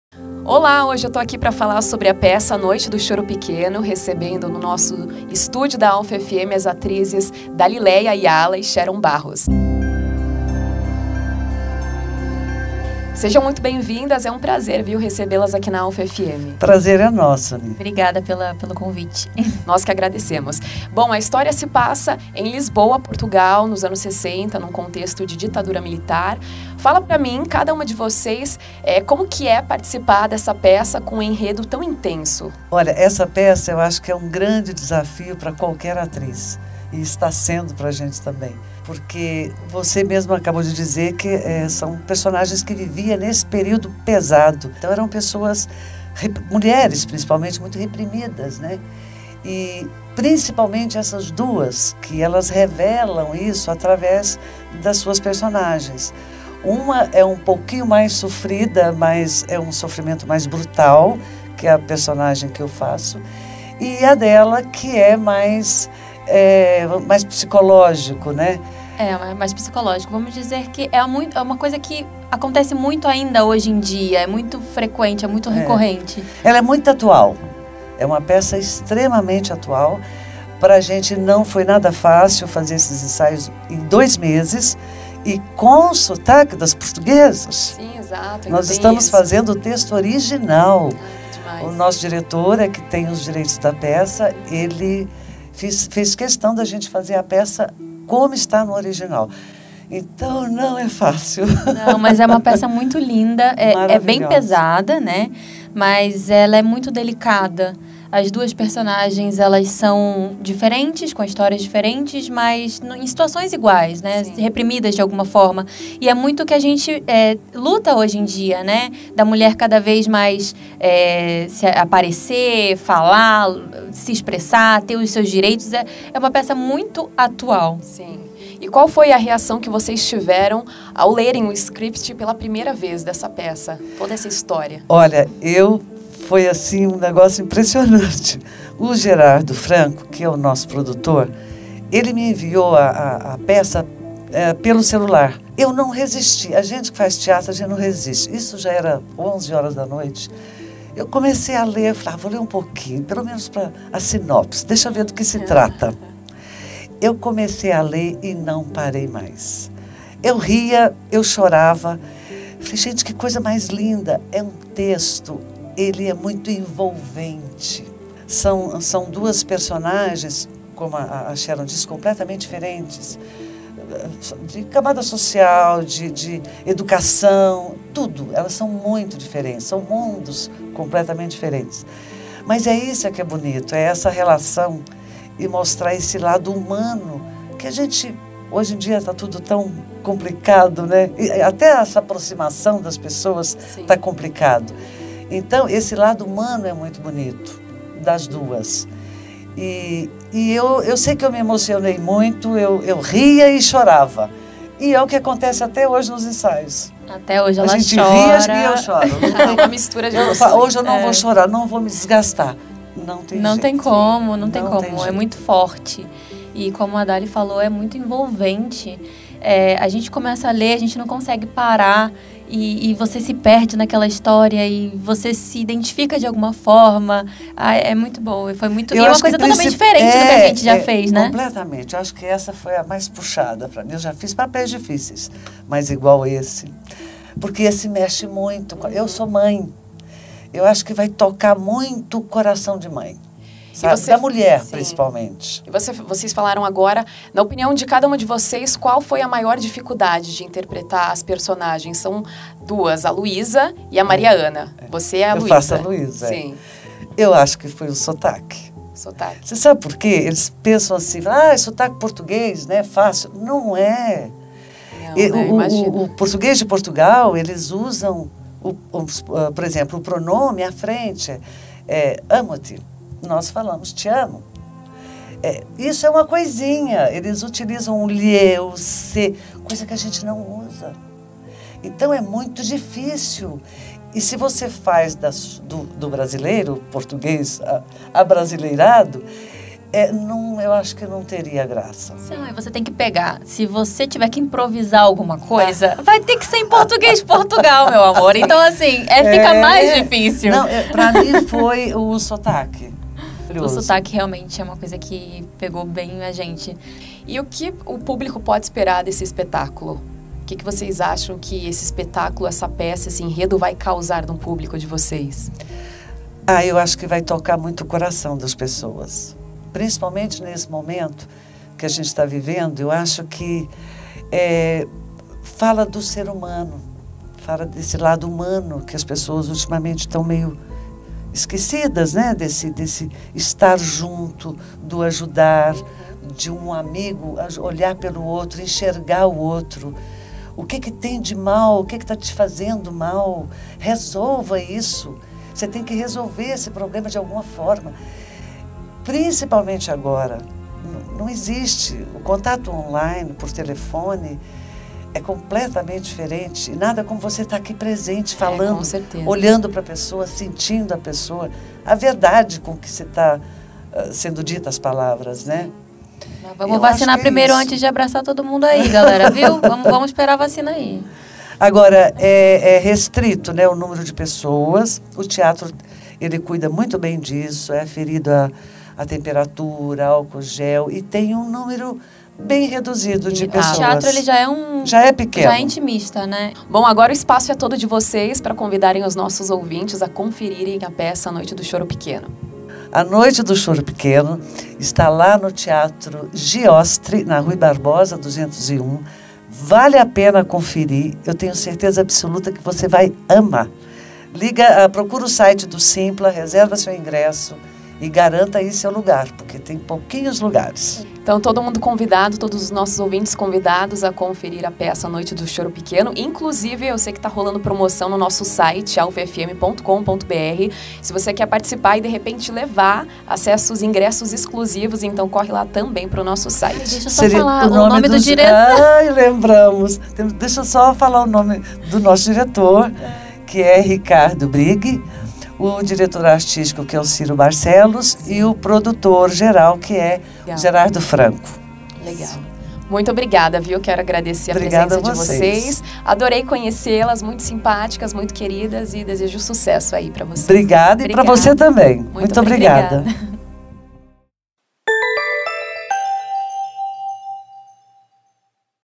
Dalilea Ayala e Sharon Barros, de "A Noite do Choro Pequeno", falam sobre a peça | Alpha FM 101.7
Conversamos com as atrizes Dalilea Ayala e Sharon Barros, as estrelas da peça “Noite do Choro Pequeno”, uma adaptação da obra portuguesa homônima. Saiba mais sobre o espetáculo assistindo nossa entrevista exclusiva.